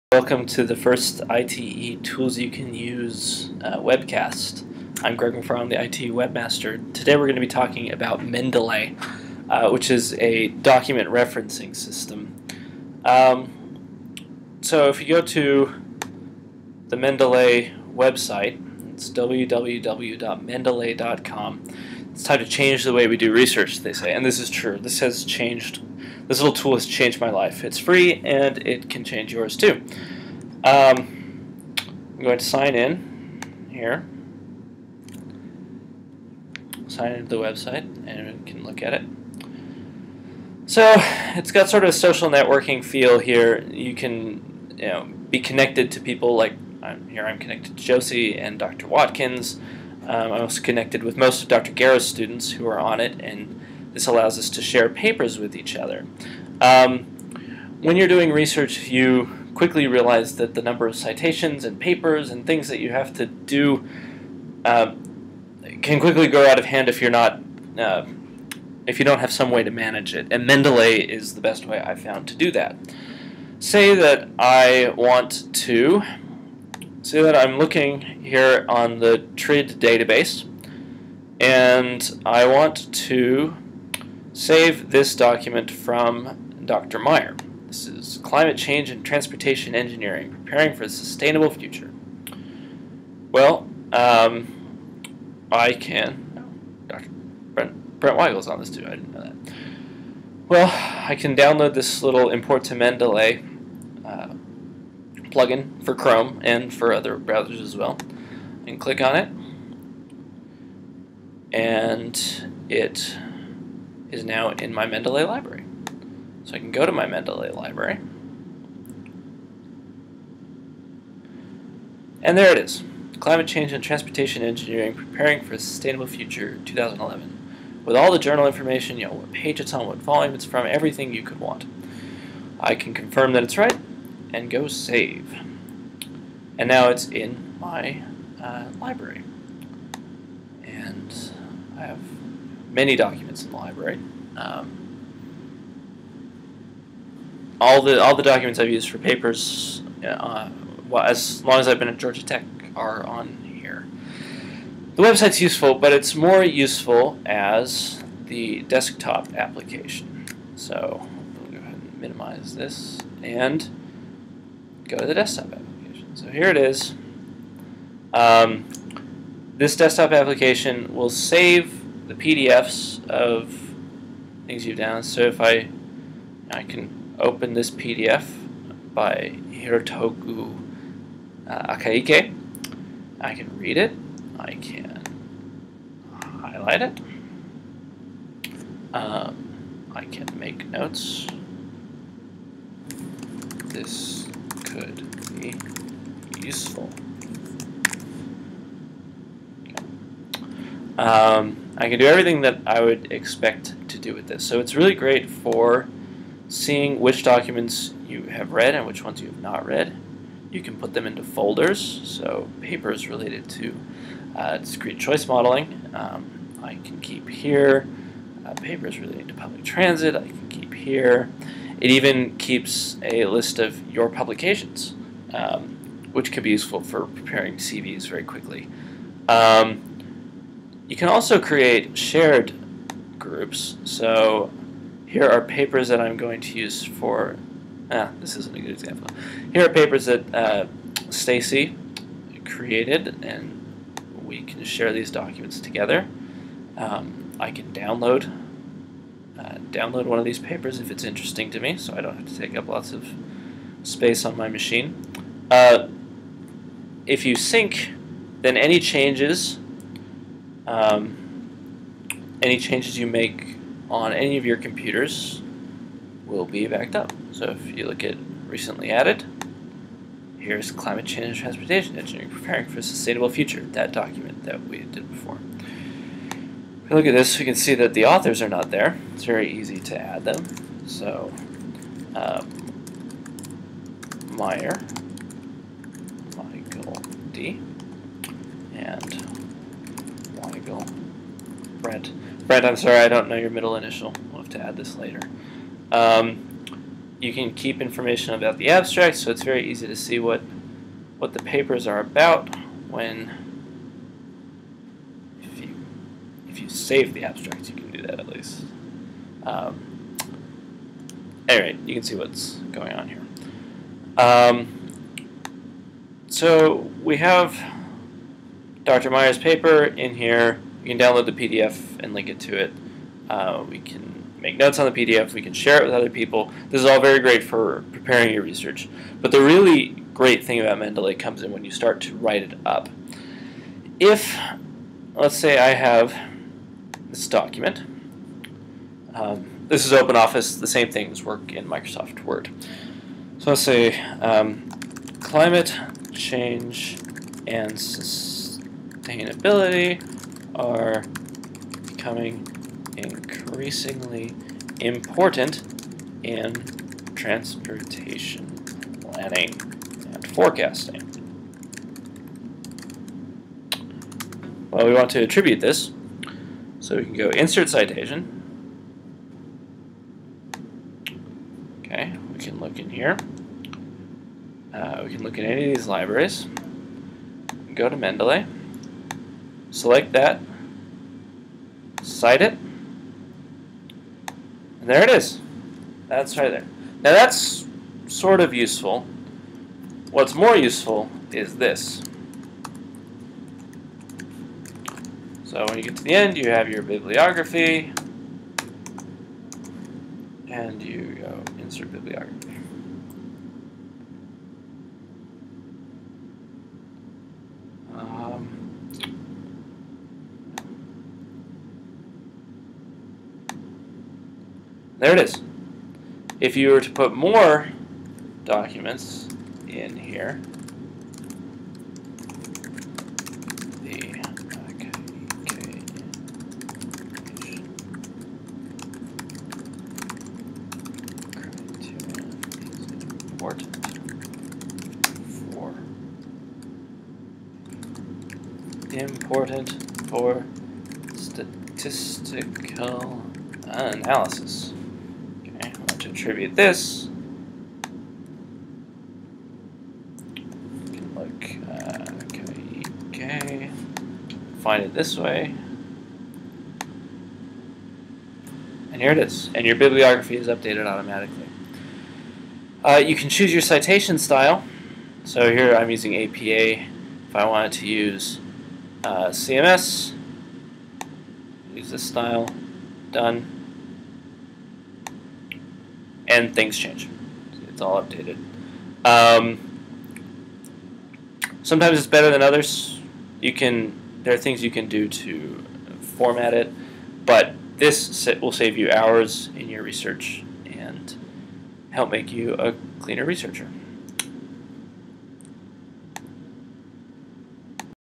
Mendeley Introduction
An intro to Mendeley from the Georgia Tech Institute of Transportation Engineers.